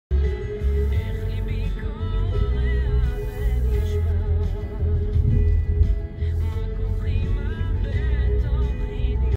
Volvo B8R Pioneer of Kavim on Route 86 on Sharet st. in Kfar Yona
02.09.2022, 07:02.

וולוו B8R פיוניר של קווים מבצע קו 86 (ממחנה בית ליד/גבעת אלונים כפר יונה לרח' האושר/האהבה (שכ' שרונה) כפר יונה) ברח' שרת/רימון כפר יונה.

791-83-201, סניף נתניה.